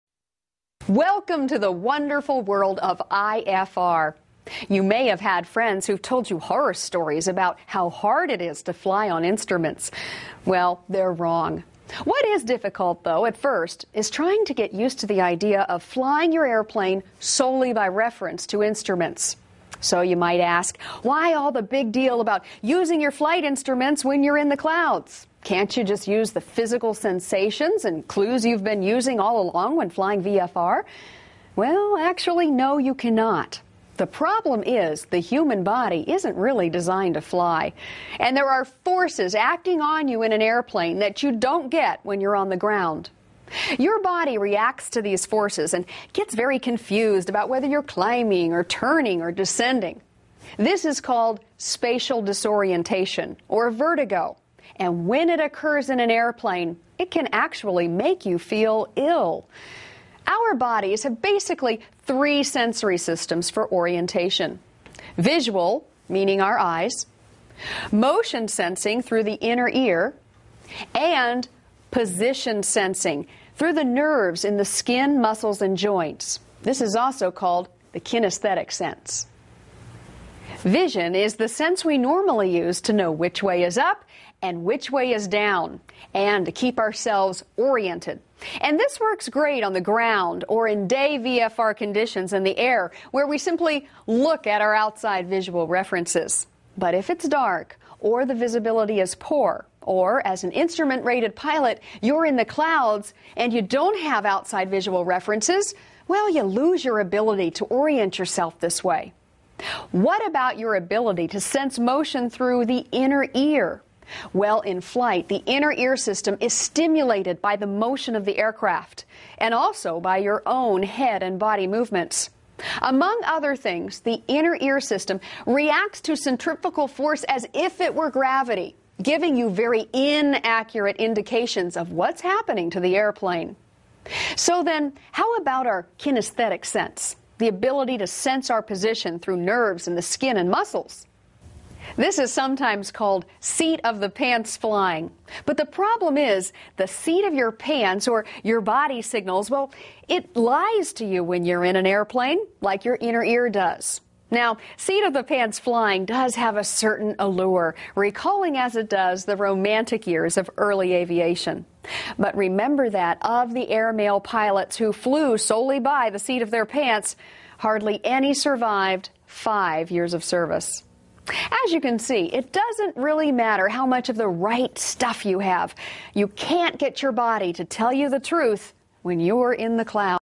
Curso Vuelo por Instrumentos #3
Why to use intruments instead of fisical senses to fly IFR
Porque usar los instrumentos de vuelo en lugar de nuestro sentidos físicos
Cd No.1
Este curso esta en venta en: cursodeaviacion.weebly.com/
*Nota: La calidad de los videos es mejor que esta...